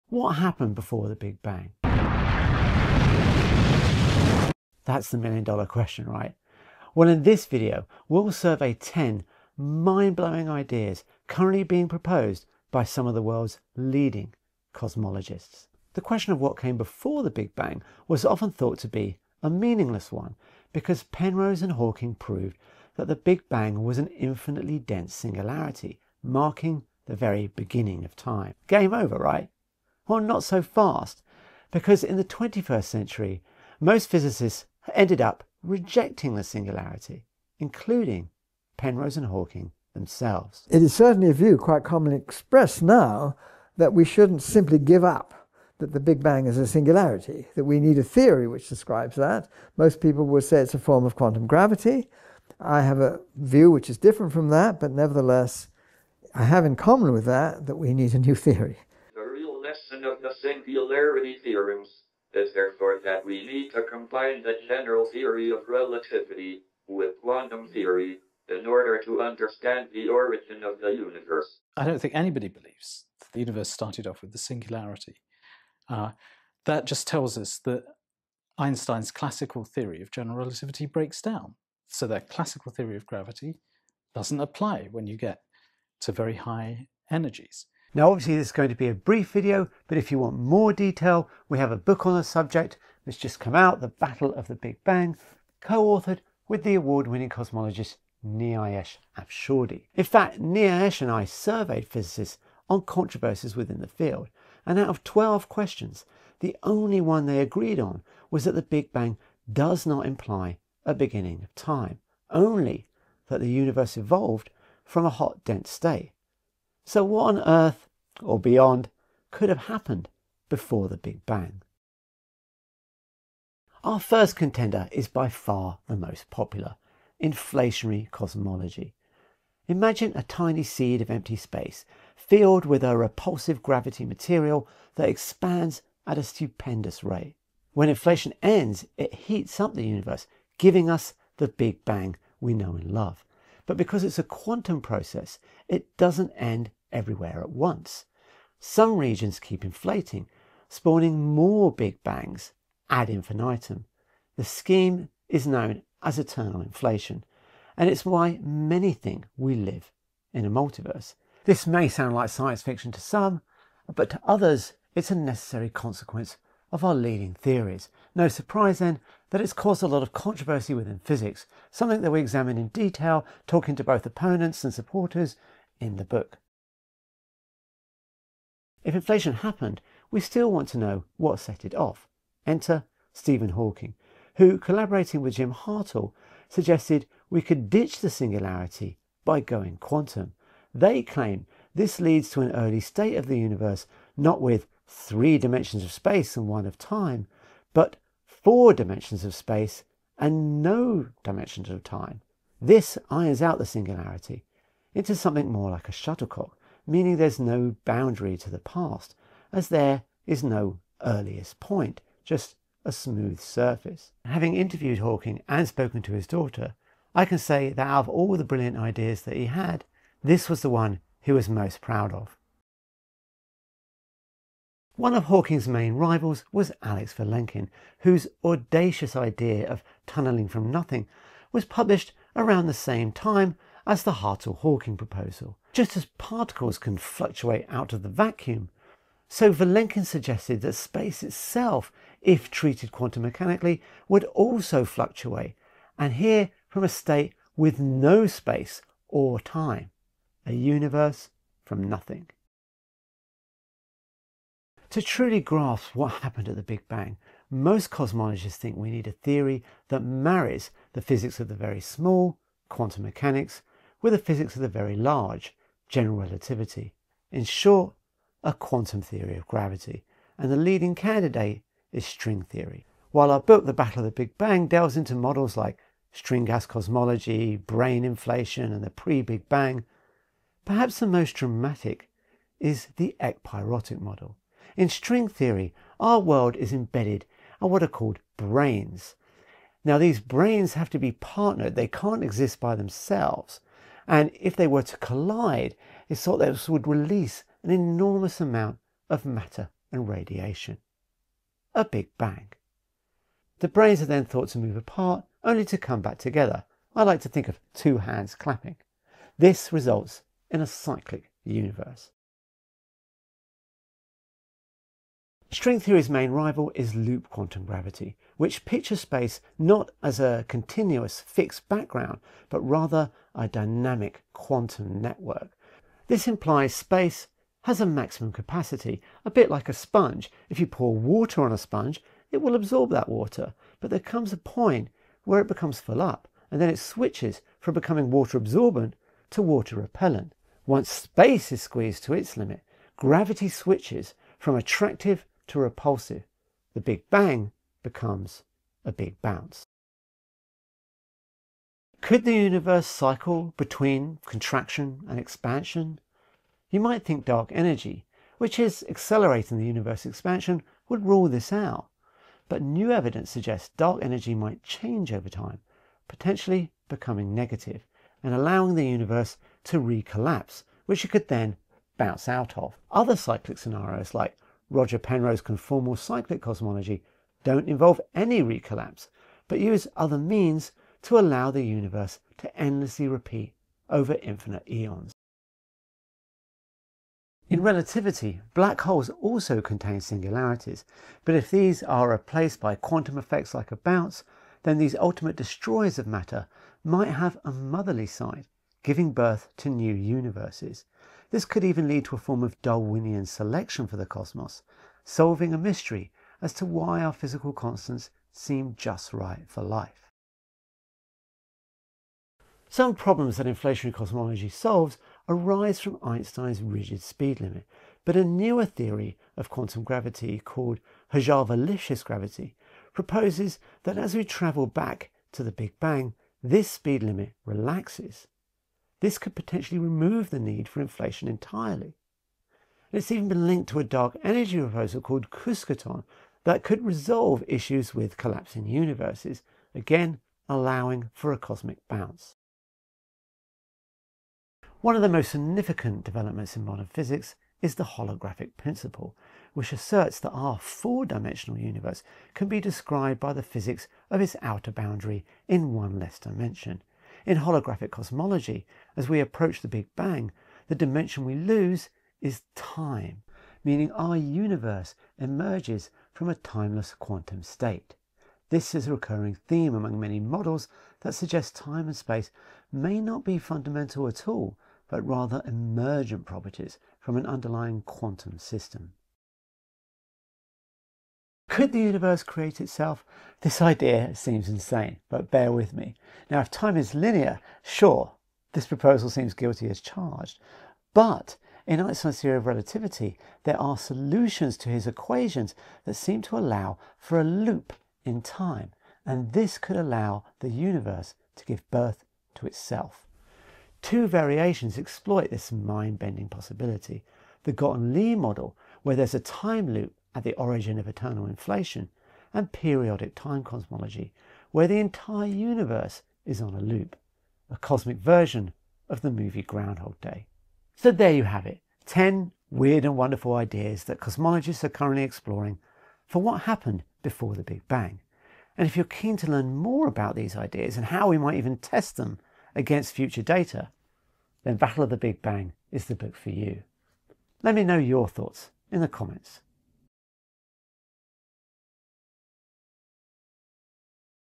What happened before the Big Bang? top 10 possibilties
What happened before the big bang?  After being asked by BBC Sky at Night Magazine to write an article about what came before the Big Bang, here’s a video…
For decades, we've been told that time, space, and matter all began with a singularity—but that idea is no longer solid. In this video, we challenge the outdated view of the Big Bang as the "beginning" by going straight to the source: Roger Penrose and Stephen Hawking. Both helped create the singularity theorems. I've interviewed both of them and here you can see that neither believe the singularity.
So if the Big Bang wasn't the beginning... what came before it?
In this video  we explore 10 radical models that attempt to answer this ultimate question, including: Penrose Conformal Cyclic Cosmology, Eternal inflation, black hole genesis, holographic cosmology, string cosmology, loop quantum cosmology, variable speed of light theory and self creating universes.
Timeline:
00:00 intro
00:22 Penrose and Hawking
2:01 eternal inflation
2:57 Hawking Hartle No Boundary Proposal
3:43 tunnelling from nothing
4:14 string cosmology
5:15 big bounce
5:50 cyclic universe
6:25 black hole genesis
6:51 varying speed of light
7:24 holographic cosmology
8:00 self creating universe